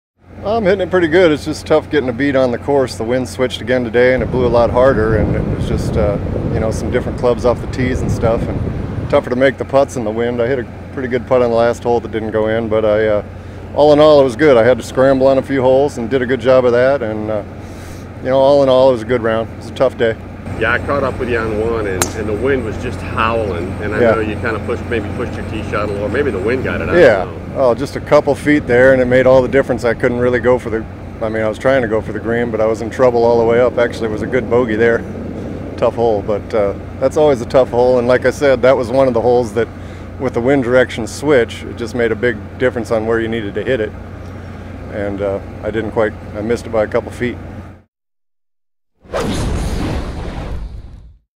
Shane Bertsch comments after Round 2 of Midwest Classic
Following his second-round 70 at the 2014 Midwest Classic, Shane Bertsch talks about the difficult wind conditions but all in all played well.